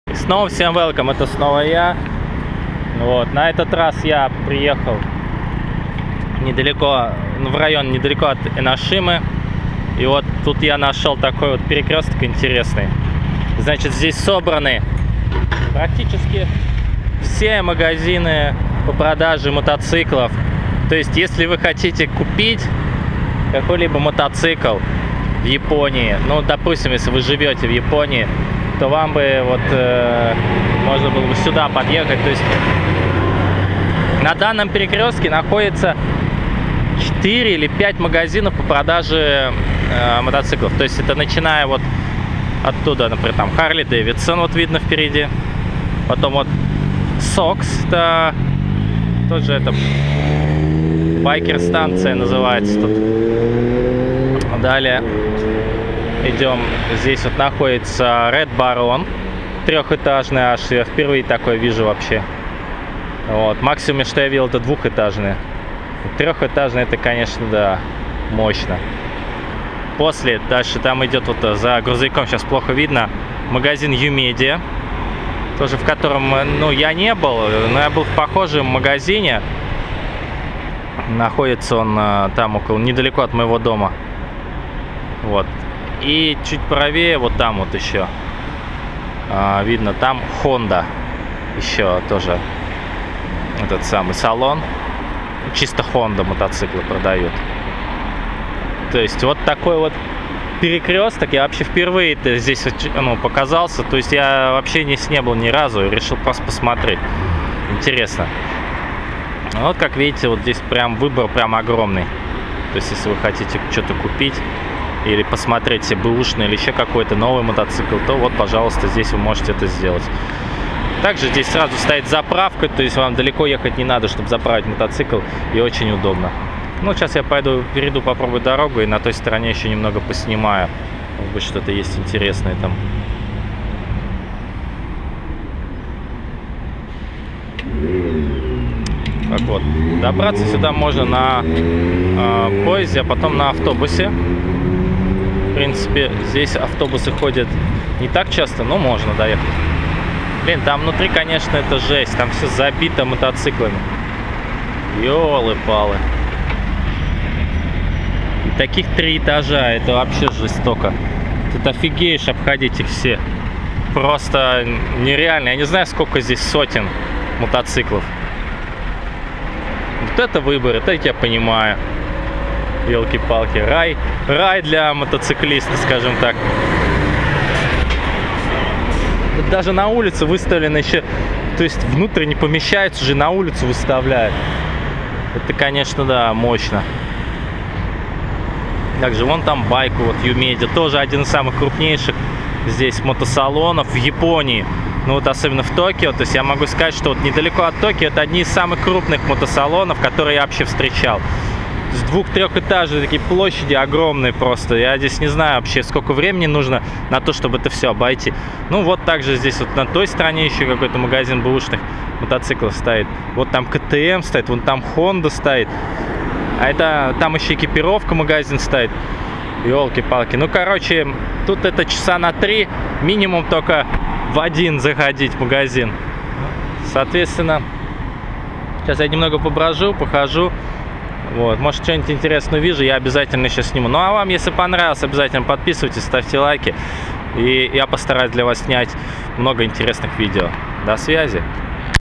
Япония. Мото мекка в Японии. Самые крупнейшие мото салоны в Японии. Весна 2016
Япония. Мото мекка в Японии. Самые крупнейшие мото салоны, магазин в Японии. Мото запчасти.
► Мой видео канал о жизни в Японии DenJP